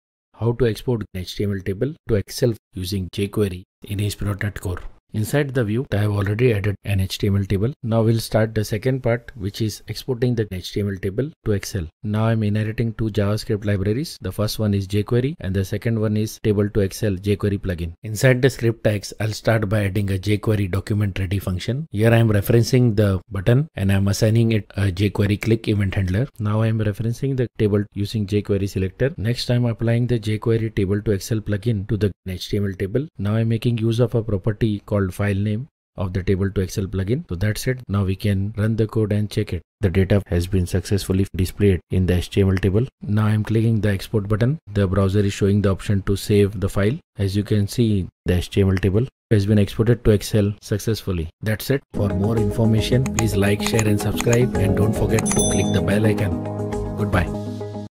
Export HTML Table to Excel using jQuery in ASP.Net Core MVC
Here Mudassar Khan has explained how to export HTML Table to Excel file using jQuery in ASP.Net Core MVC.

The HTML Table will be exported (converted) to Excel file using the jQuery table2excel plugin in ASP.Net Core MVC.